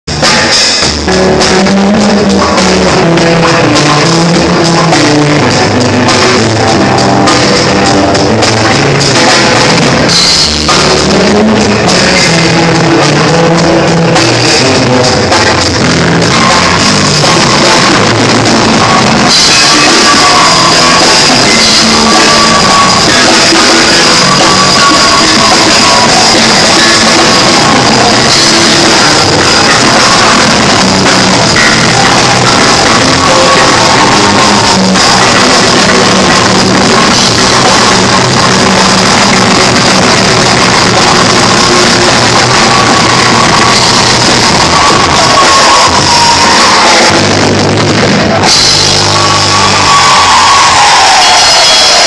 Pastor Isaiah displays on Bass Guitar in 2012
2012 bass and drum solo by Pastor Isaiah Philip Boateng and Abigail